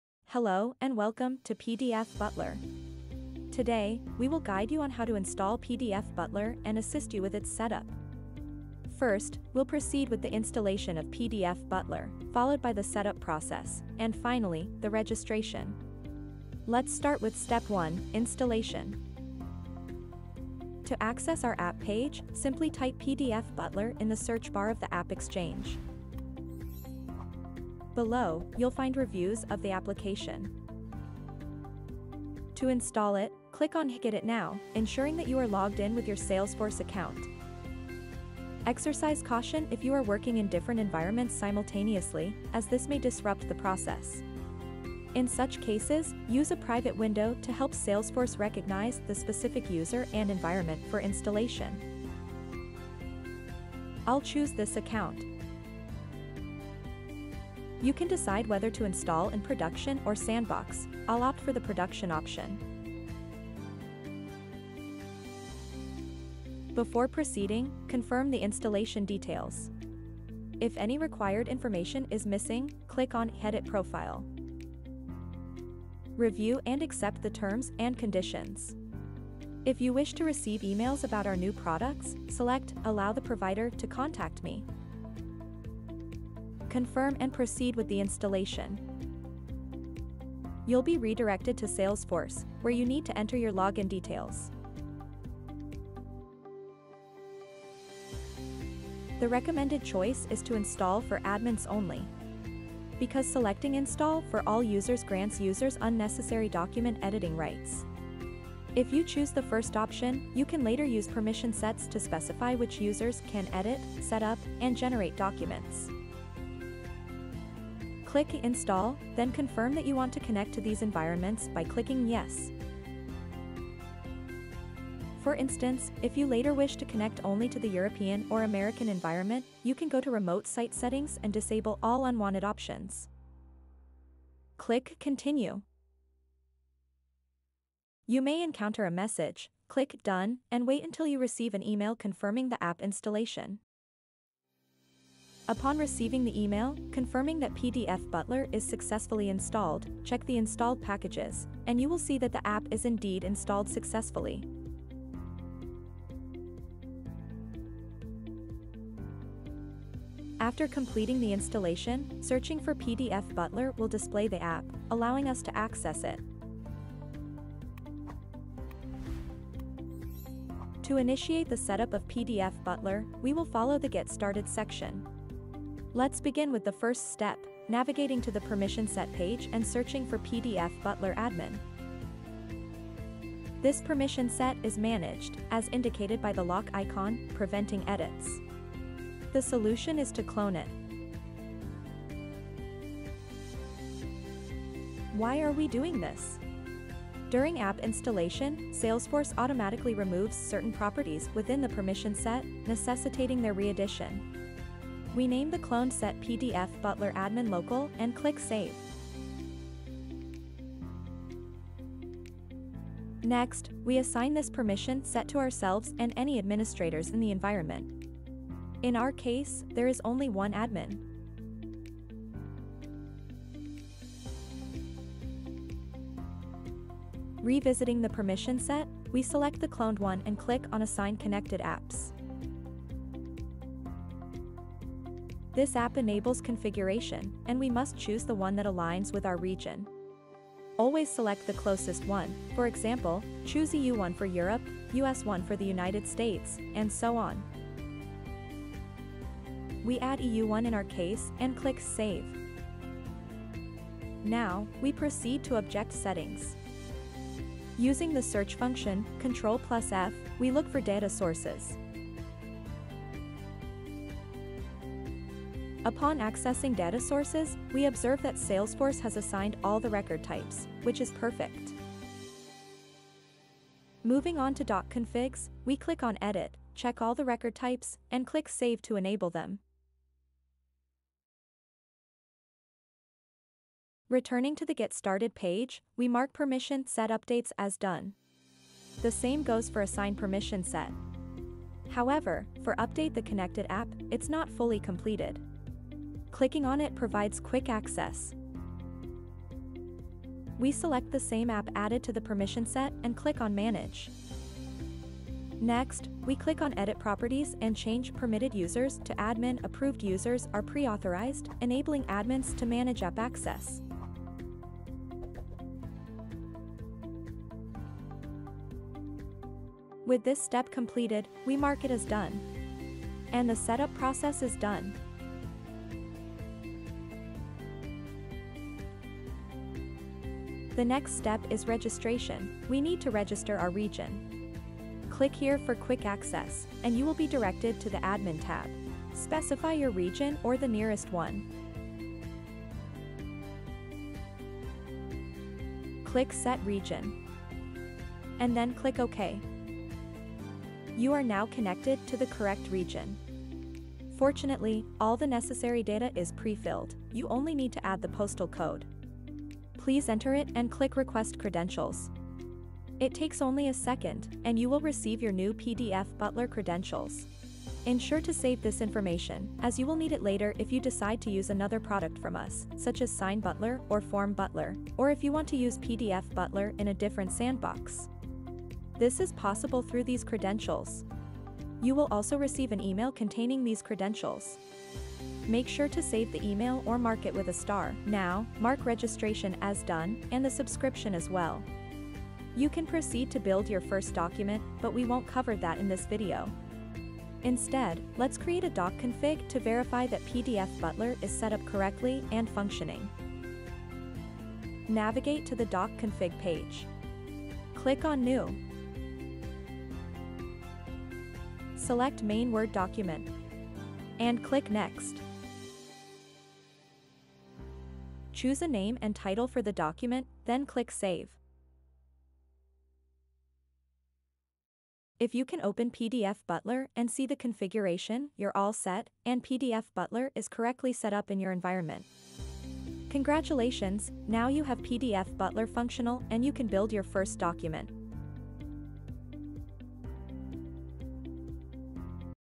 GET STARTED - Install, setup & register
Install from the AppExchange and configure PDF Butler in 10 minutes. All that needs to be done to get started!